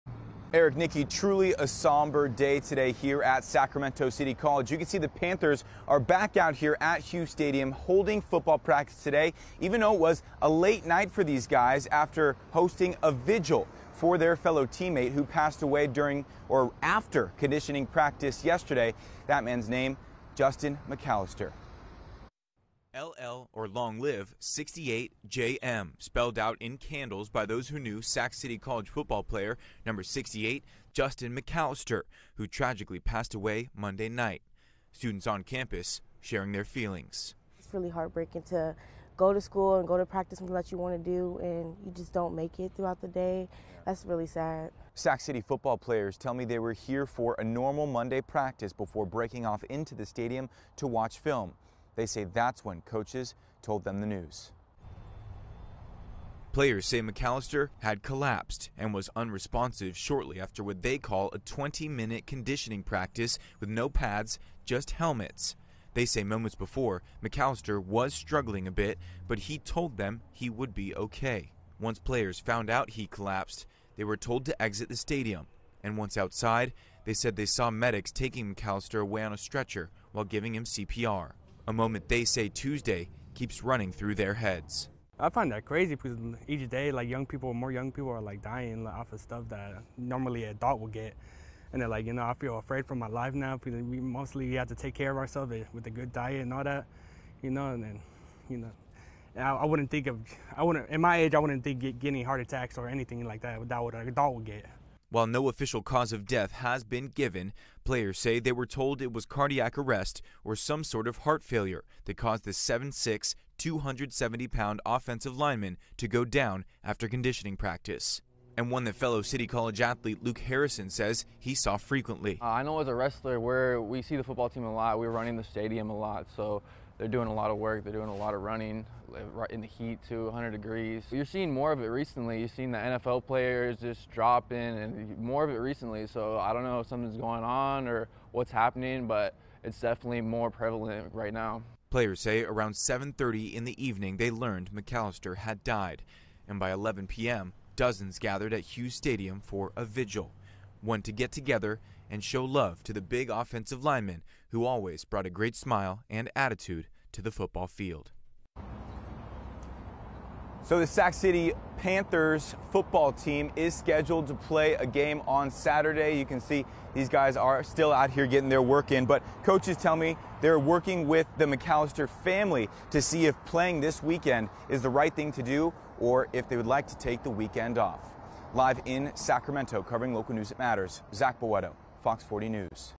Sacramento City College football player dies after conditioning practice
Sacramento City offensive lineman Justin McAllister died after collapsing following a routine conditioning practice.